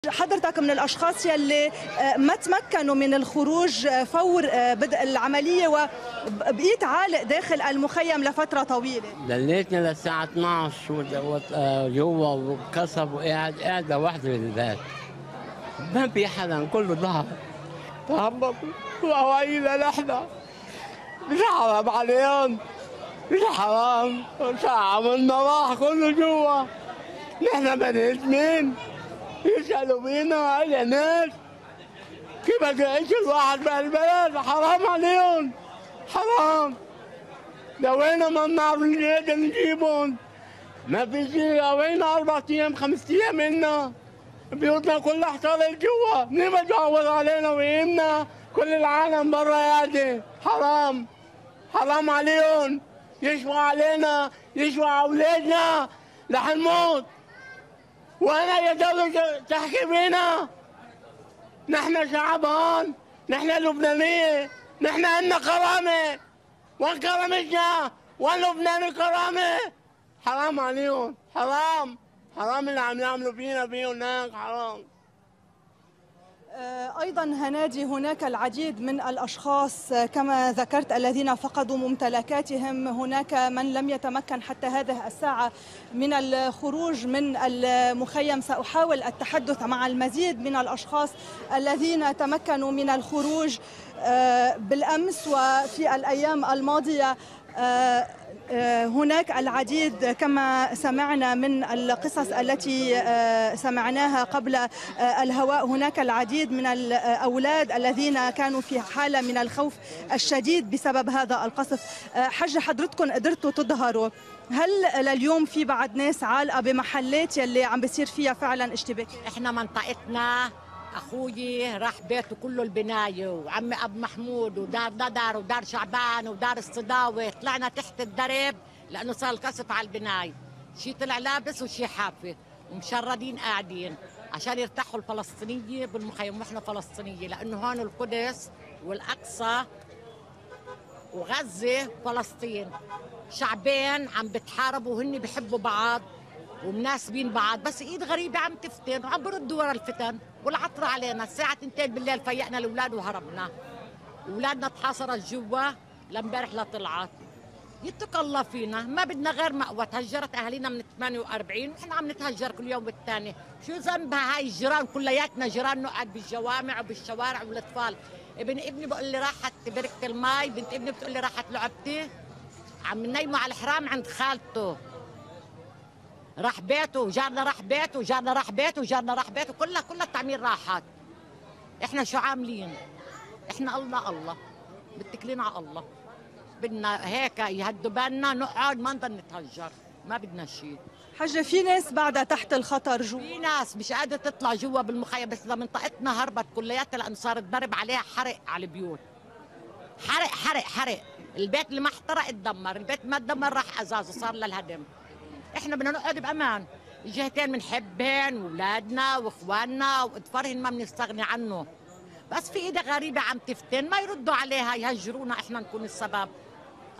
"مش حرام عليهم".. شهادات مؤلمة لنازحي مخيم عين الحلوة
أكثر من ألفي نازح من مخيم عين الحلوة بسبب المعارك الدائرة في أحيائه.. شاهد كيف وصف أهالي المخيم النازحون الاشتباكات العنيفة بين حركة فتح وبعض التنظيمات الإسلامية.